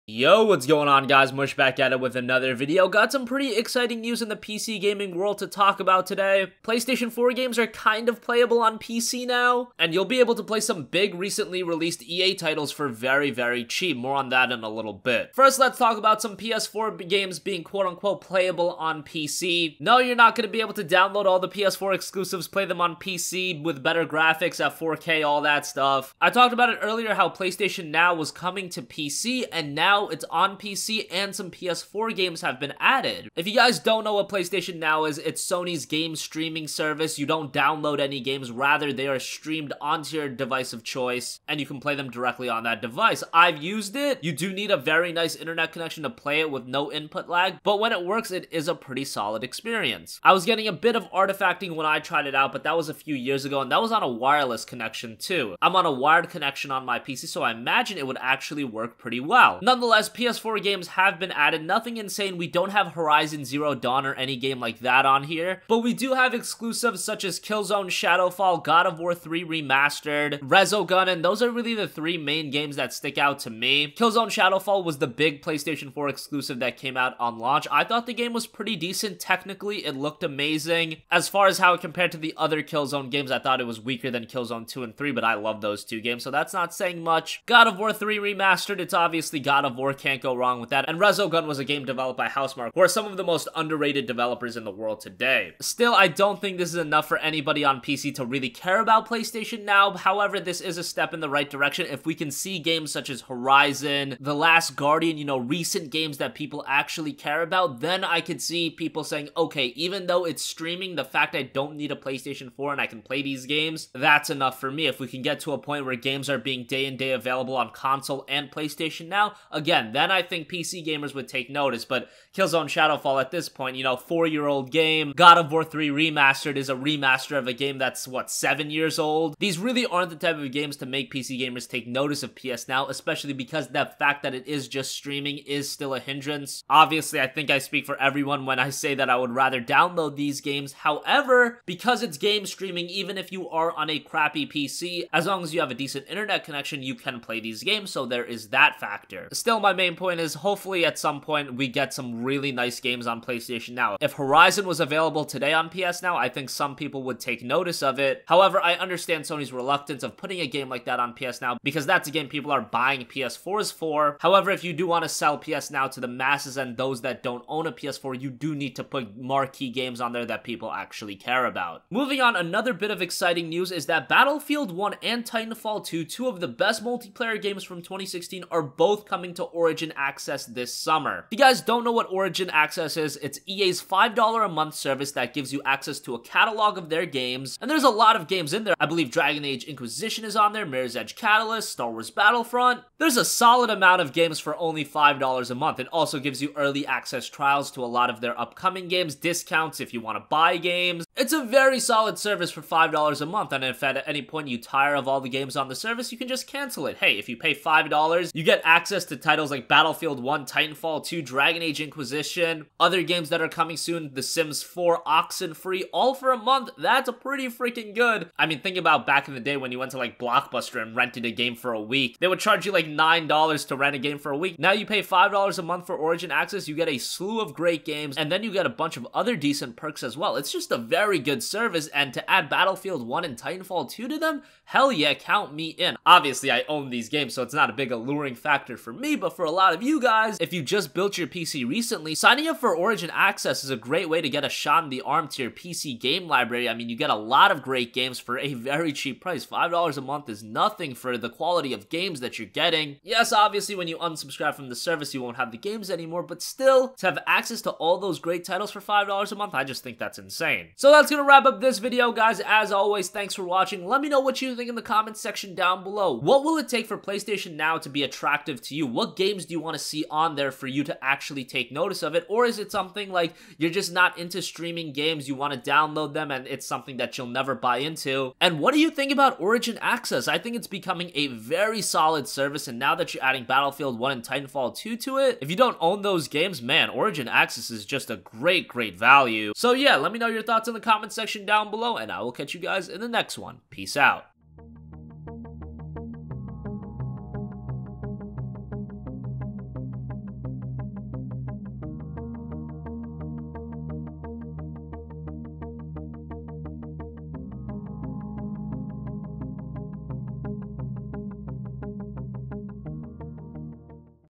PS4 Games on PC and Play Battlefield 1 SUPER Cheap!
So what are your thoughts on PS4 Games being on PS Now? Will you try out Origin Access to play Battlefield 1 and Titanfall 2?